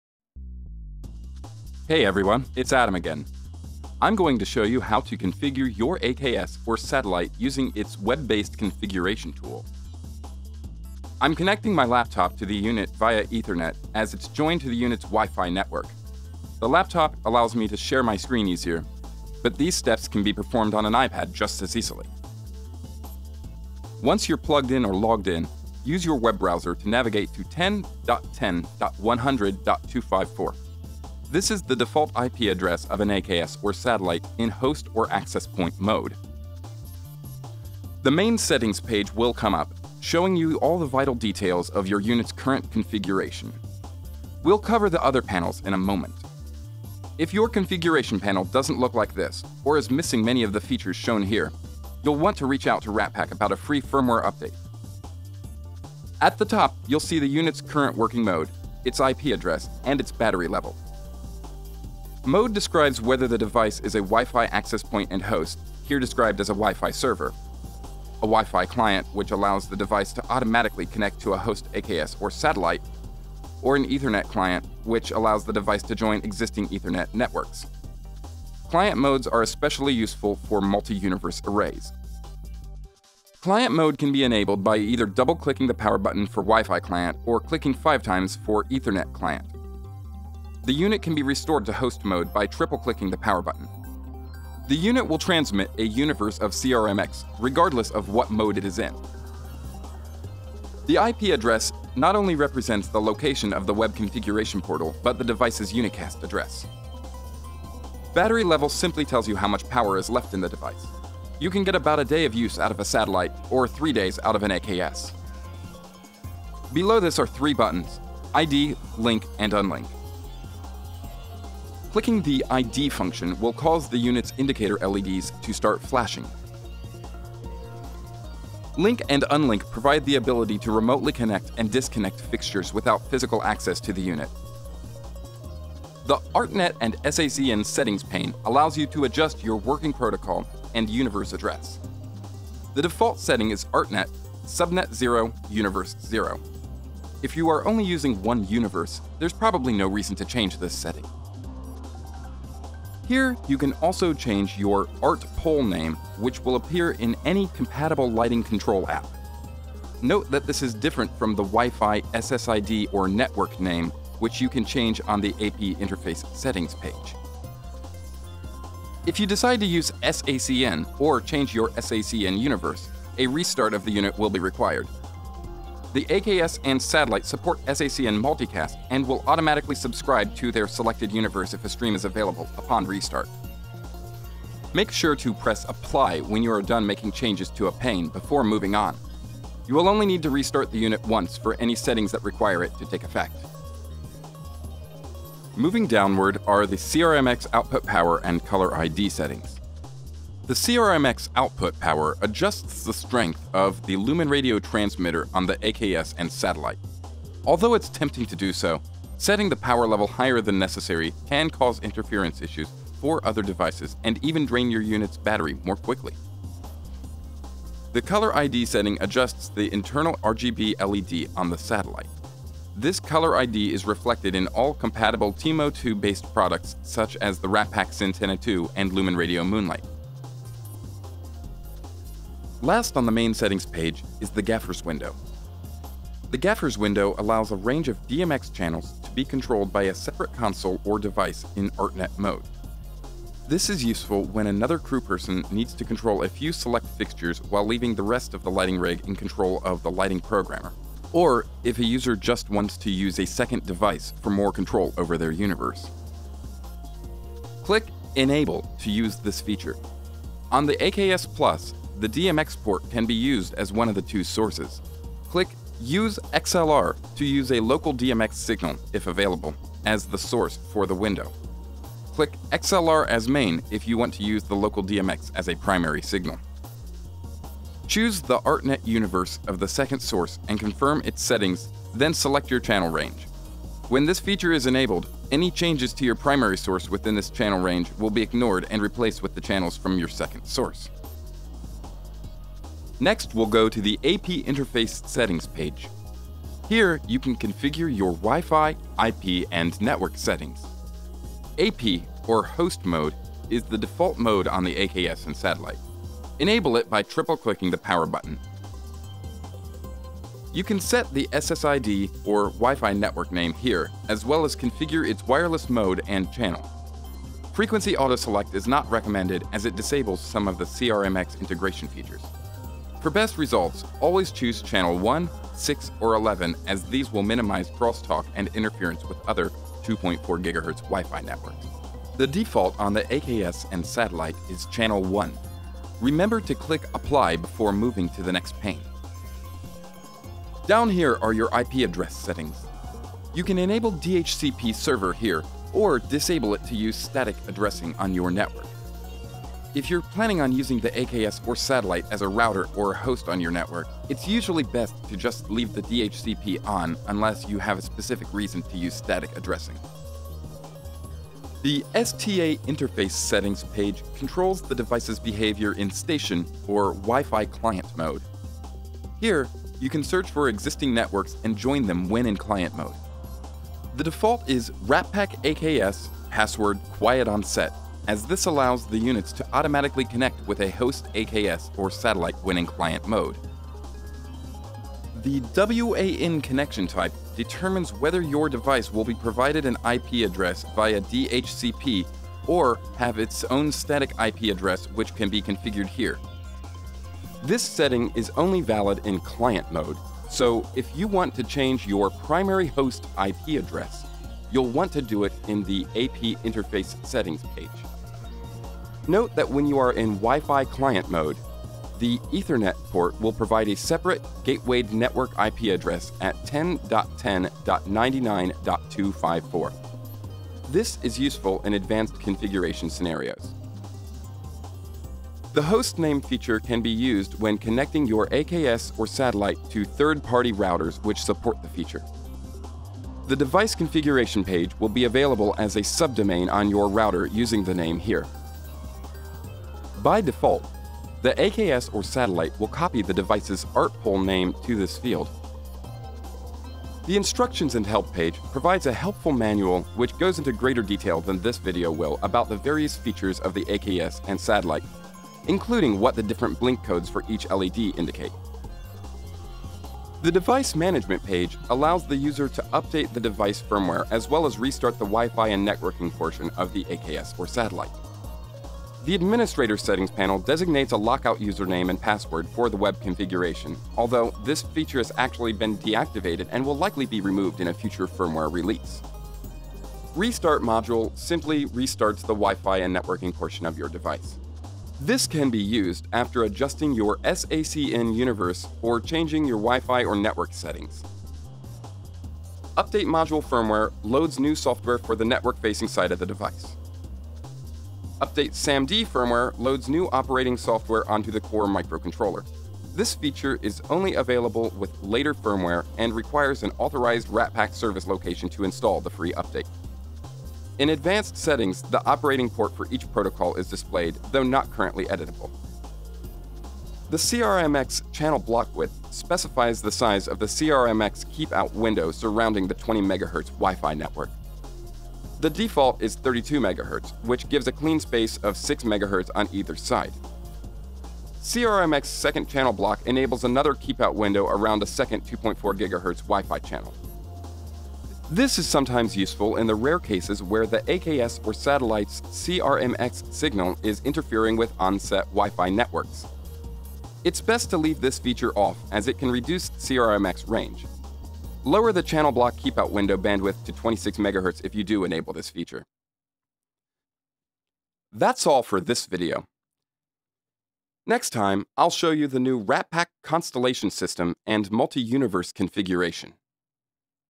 AKS Satellite Web Configuration Instruction
This video is the second in a series of videos about the AKS and Satellite by Ratpac. This particular edit is rough and will be replaced.